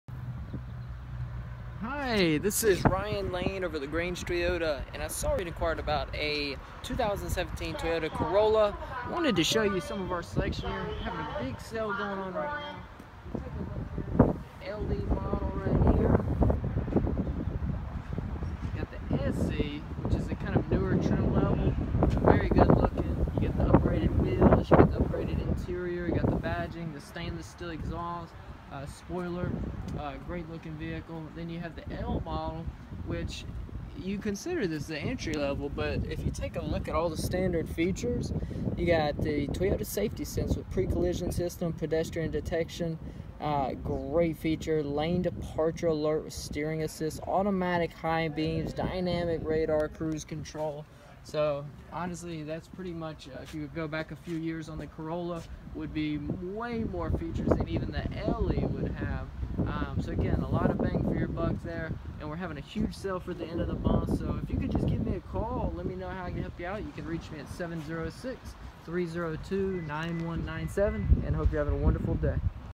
2017 Toyota Corolla Review
Demonstration of 2017 Toyota Corolla by Ryan Lane at LaGrange Toyota